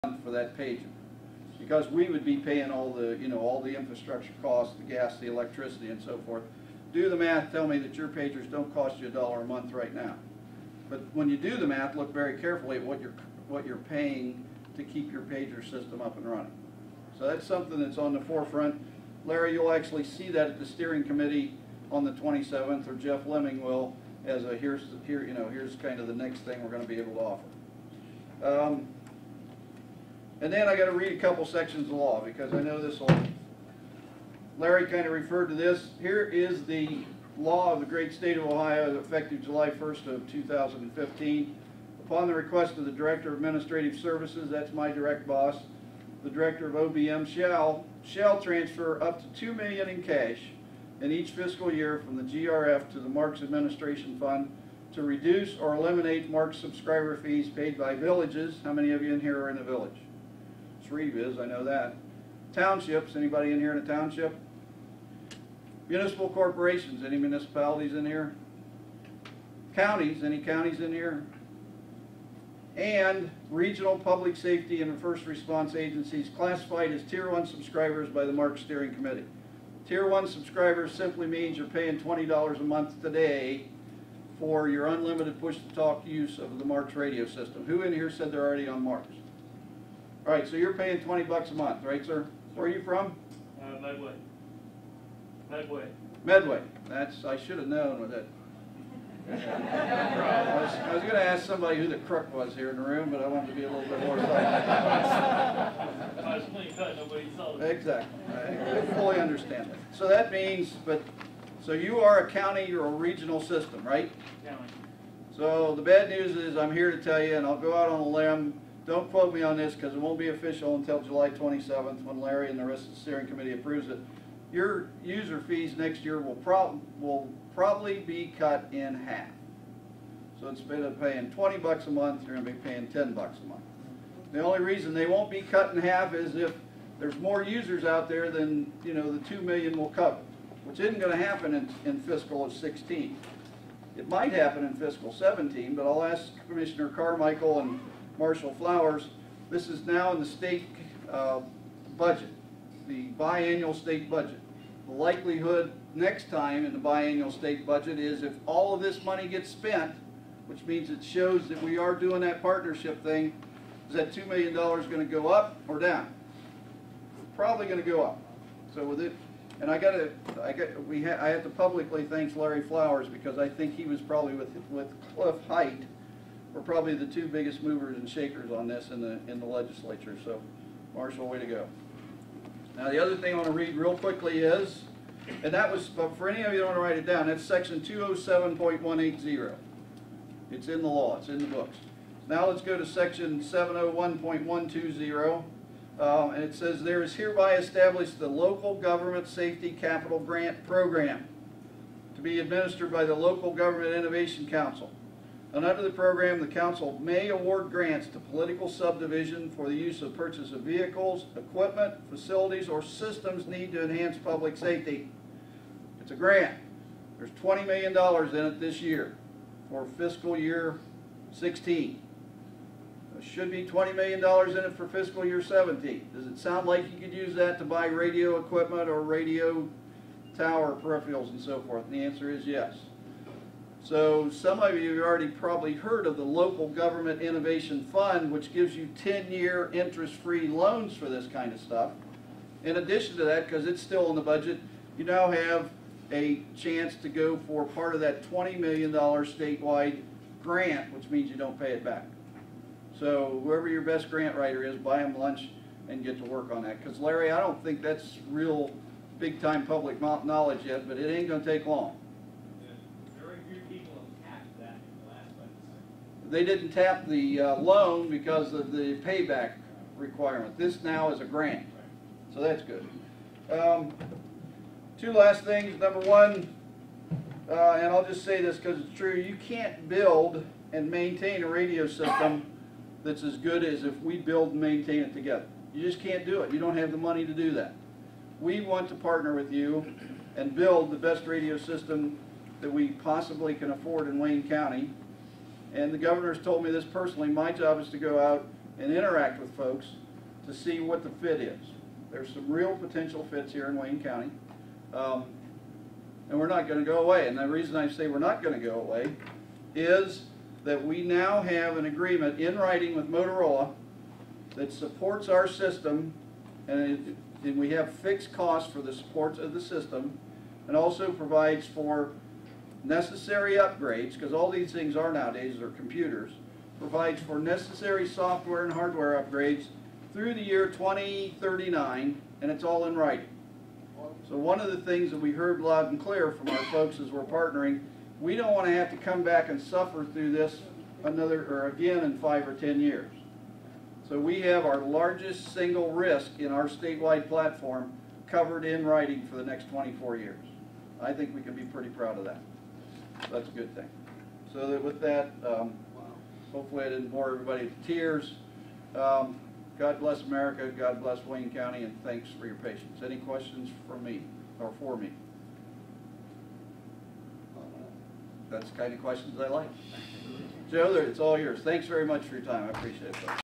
Wayne County Communications Meeting 8
Darryl Anderson, program director for the Multi-Agency Radio Communications System, talks about some of the system's infrastructure and about a new paging system being introduced.